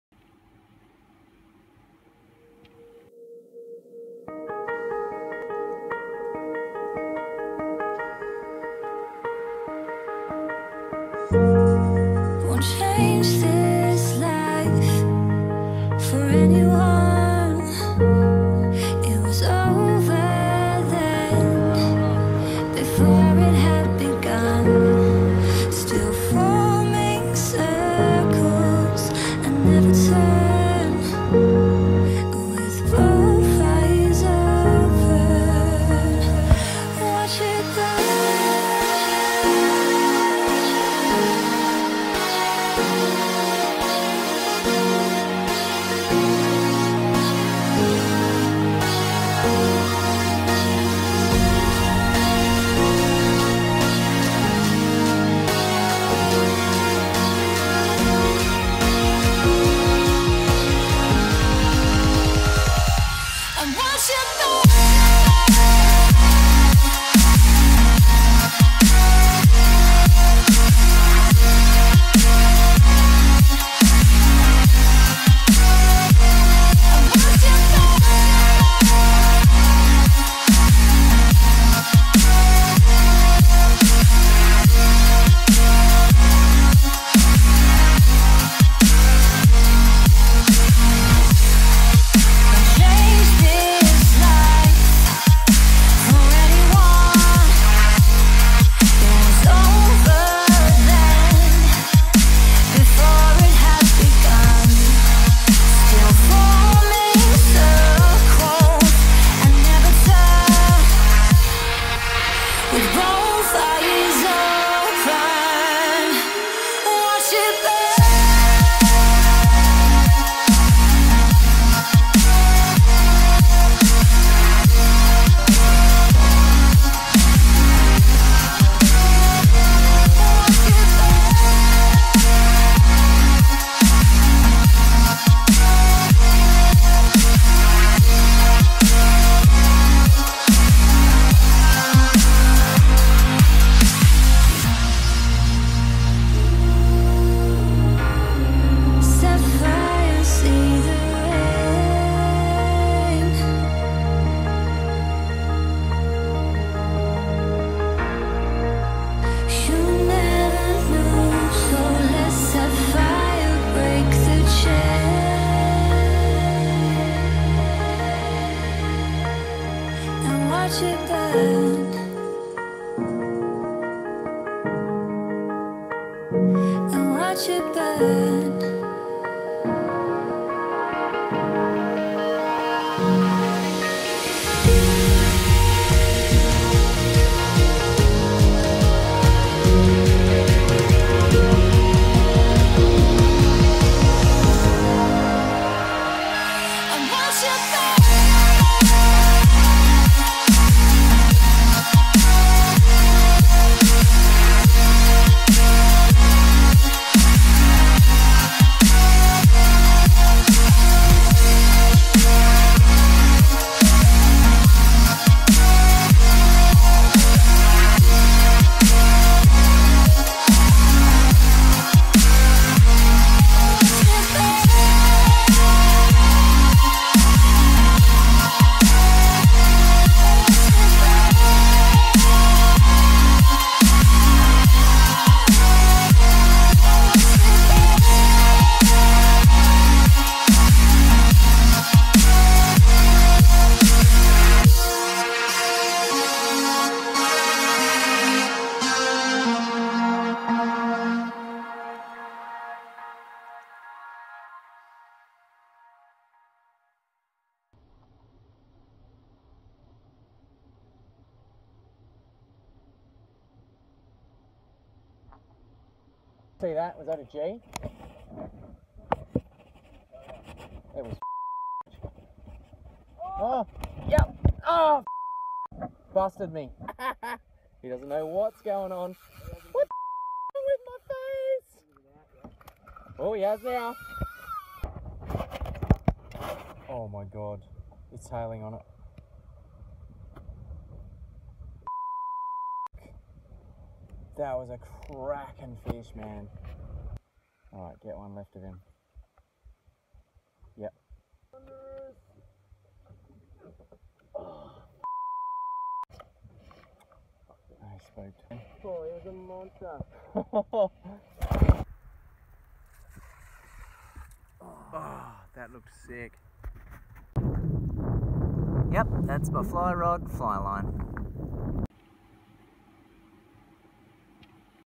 Determination
2 fly addicts tackle the remote Pilbara in a leaky 40 year old boat that only runs on 3 out of 4 cylinders. Pristine flats as far as the eye can see and not a sole in sight.flyfishing camping flatsfishing bluebastard Islands fishing moondoglabs #@aquasoul #@capetocape#Pilbara